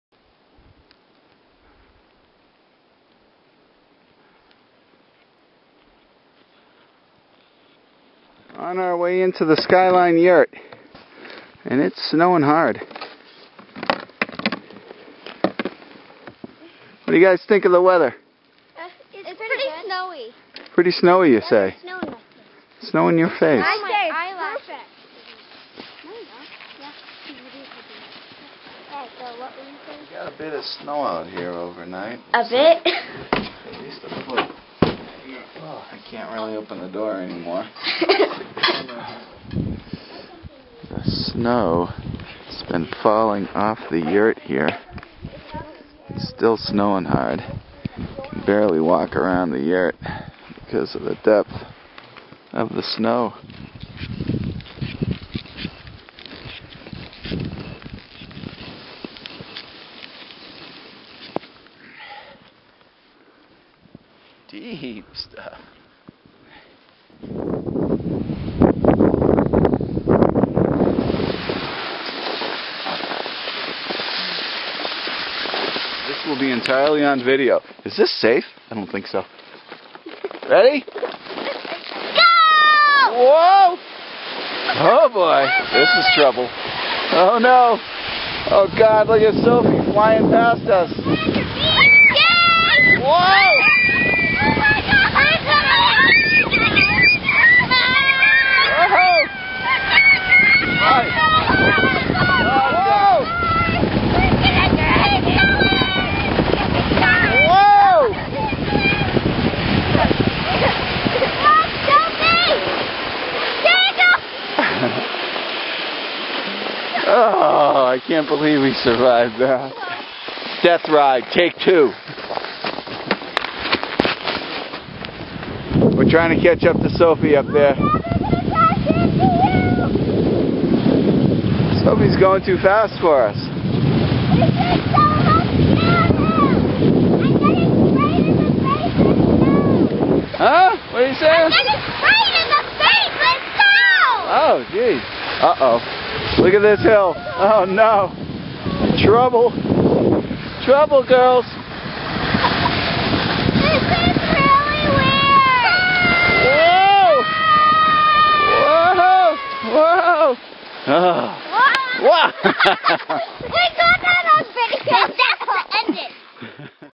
Skyline yurt trip movie
A family ski trip to the Skyline yurt in the backcountry of Idaho's Boise National Forest